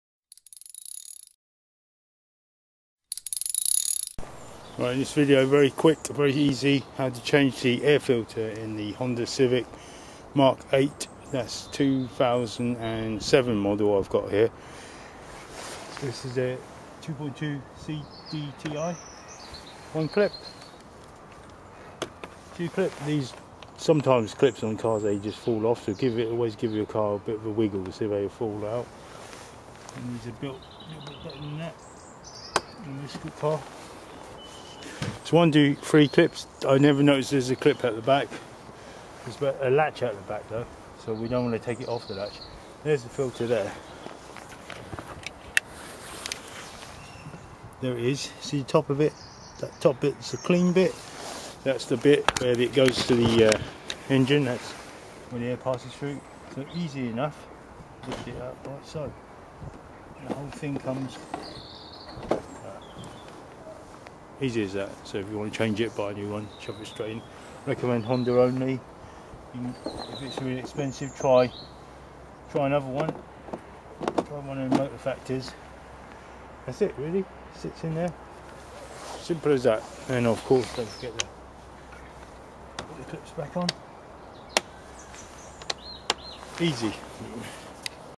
How to Change the Air Filter - Honda Civic 2.2 CDTI MK8 2006-2011
The air filter on the Honda Civic 2.2CDTI MK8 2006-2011  is designed to be easily removed and changed.